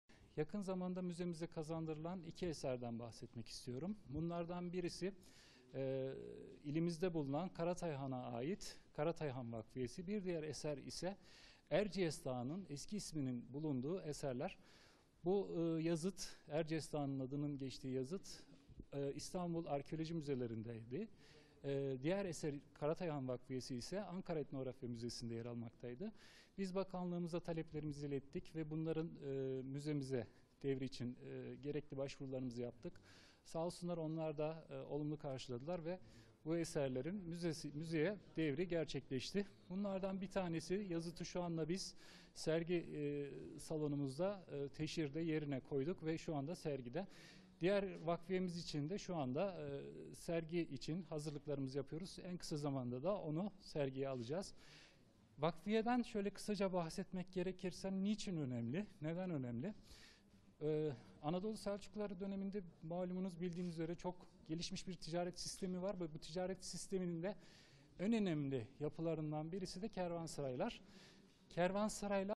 Erciyes’in isminin geçtiği 2 bin 800 yıllık kitabe Kayseri’ye getirildi
İSTANBUL Arkeoloji Müzesi’nde uzun yıllardır sergilenen ve üzerinde Erciyes Dağı’nın ikinci ismi ‘Hara Hara’ yazılan 2 bin 800 yıllık Hisarcık Kitabesi, Kayseri’ye getirilerek, Arkeoloji Müzesi’nde sergilenmeye başladı.

Müzeler Haftası nedeniyle yakın zamanda İstanbul Arkeoloji Müzesinde sergilenen 2 bin 800 yıllık Hisarcık Kitabesi ve Ankara Etnografya Müzesinde bulunan Karatay Han Vakfiyesi, Kayseri Arkeoloji Müzesi’ne getirildi. İl Kültür ve Turizm Müdürlüğü envanterine kaydolan eserlerden 2 bin 800 yıllık Hisarcık Kitabesi sergilendi. Kitabe hakkında açıklama yapan Ankara Üniversitesi Dil ve Tarih-Coğrafya Fakültesi Arkeoloji Bölümü Öğretim Üyesi ve Kültepe Kazı Başkanı Prof. Dr. Fikri Kulakoğlu, "Erciyes'in isminin geçtiği kitabe 1900'lü yılların başında Kayseri'de müze olmadığı için İstanbul'a gönderilen bir eserdir. Bu eser İstanbul Arkeoloji müzesinde sergileniyordu. Ancak, müzede çok göze dokunan bir yerde değildi. Eser Kayseri tarihi için önemli. En başta Kayseri'nin simgesi olan Erciyes'in ismi bu kitabede geçiyor. Erciyes'in adını biz 4 bin yıl önceden biliyoruz. Kültepe tabletlerinde ‘aşkaşipa’ diye geçen bir dağ ismi. Aynı zamanda tanrı olarak tapınılan bir dağ. Taşın ait olduğu 2 bin 800 sene öncesinde ise ‘Hara Hara’ olarak isimlendiriliyor. Kayseri tarihi aydınlatması açısında çok önemli isimdir. Eserin olduğu dönemde Kayseri bulunduğu yer Tabal krallığı olarak biliniyor. Bu kitabede hiyeroglif yazı ile o dönem Kayseri ve Kapadokya'da çevresinde Tabal krallığına ait beyliklerden bahsediyor. Bu kitabeyi Kayseri Arkeoloji Müzesine kazandıran bakanlık olmak üzere tüm yetkililere teşekkür ederim" diye konuştu.

‘BAKANLIĞA İLETTİĞİMİZ TALEP İLE KAYSERİ’YE KAZANDIRILDI’

Özel koruma ve sergi alanı oluşturduktan sonra ziyarete açılacak Karatay Han Vakfiyesi hakkında açıklama yapan İl Kültür ve Turizm Müdürü Şükrü Dursun ise, "Yakın zamanda ilimizde bulunan Karatay Han'a ait 'Karatay Han Vakfiyesi' ve Erciyes Dağı'nın eski isminin bulunduğu kitabe Bakanlığımıza ilettiğimiz talep neticesinde Kayseri'ye kazandırıldı. Erciyes'in isminin geçtiği kitabeyi Arkeoloji Müzemizdeki teşhir yerine koyduk. Vakfiye için ise sergi hazırlıklarına başladık. En kısa zamanda vakfiyeyi sergiye alacağız. Bu vakfiye çok önemlidir. Burada Selçuklu döneminde çok yaygın olan kervansaraydaki düzen hakkında bilgiler yer almaktadır" ifadelerini kullandı.